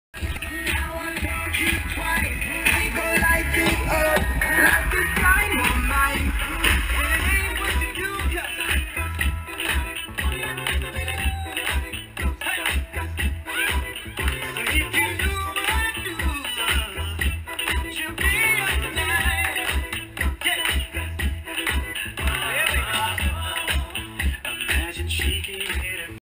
Little Kid Has Vegas Dance Floor To Himself
They had a dance floor set up outside of New York New York. The only one dancing was this little boy, he couldn't have been older than 7 or 8. 11:00 pm.